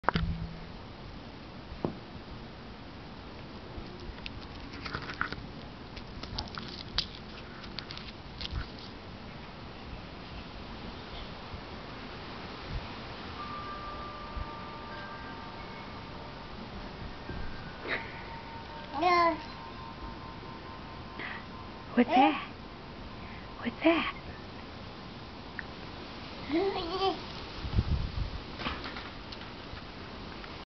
He Ran Before He Could Walk!
My son does this funny crab man crawl where he looks like he's walking, only bent over at the waist with his hands on the ground.  Better yet, he RUNS like this, and he's fast!  I've been trying to get video for a few weeks of this, but you never know when it's going to happen.  Today I bribed him with candy and he did it a little bit.

(sometimes, if he goes too fast, he'll mess up his center of gravity and totally faceplant)